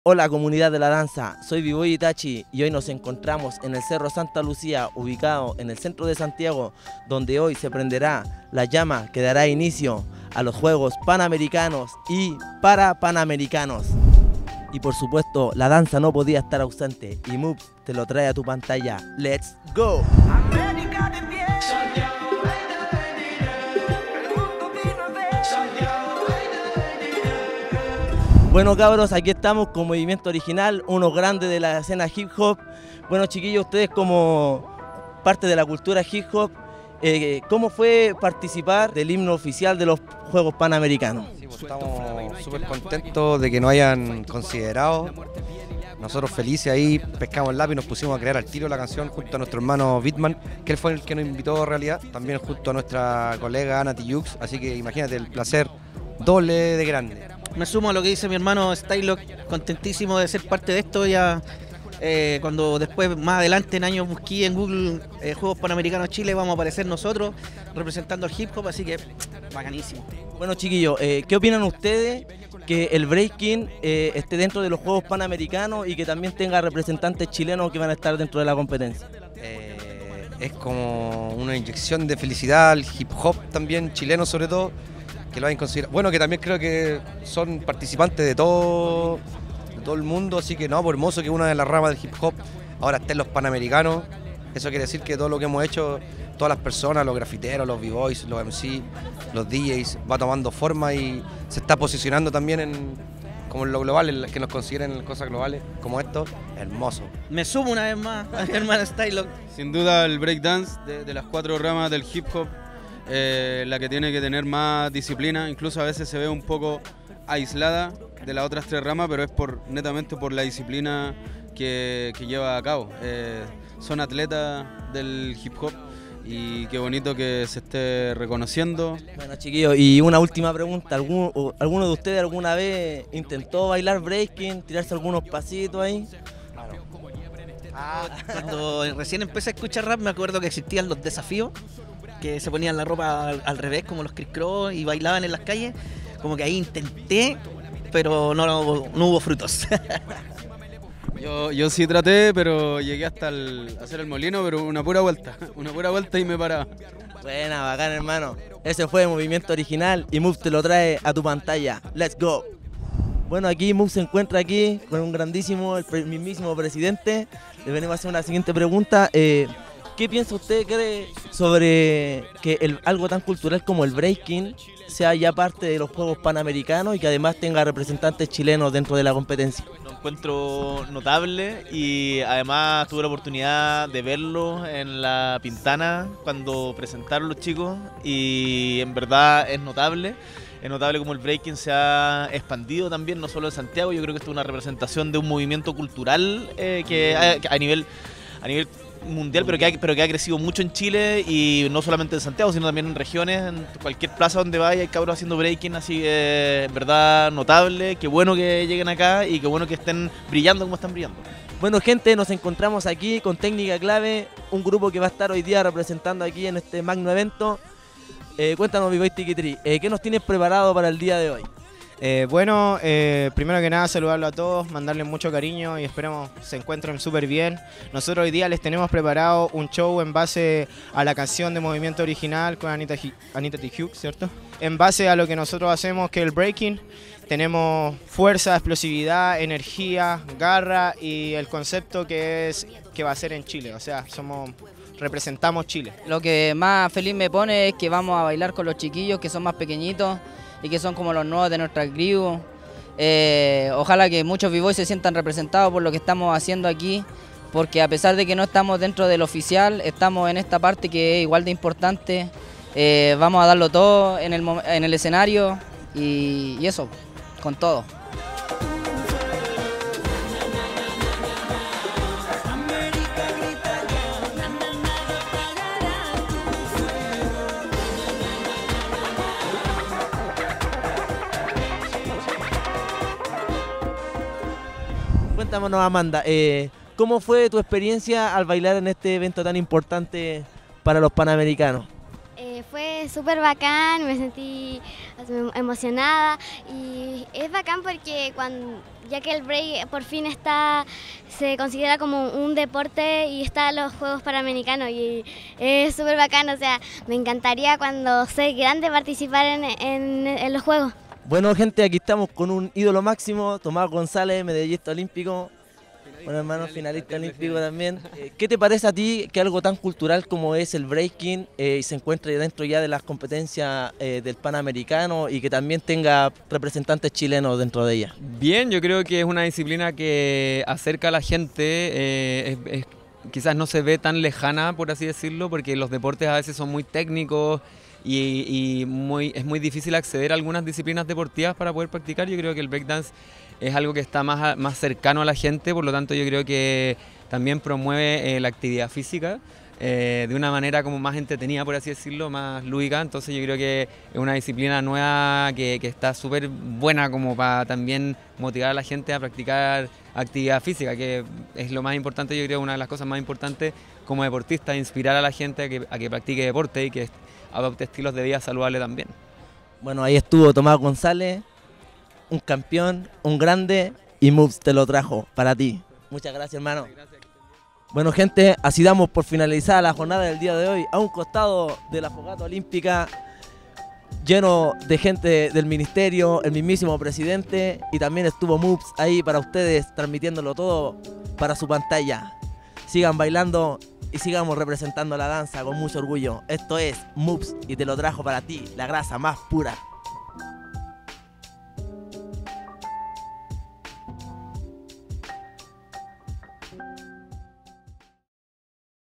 MUBS | El Fuego Panamericano Santiago 2023
🔹Nota🔹

Estuvimos en el encendido de la llama Panamericana y en donde el Breaking se hizo presente 💪

Cada vez queda menos para ver al baile en lo más alto del deporte en nuestro país 🇨🇱🏆

Déjanos en los comentarios tu opinión 🙌

🟪 Síguenos y entérate de cómo se vive la danza en esta parte del mundo 🟦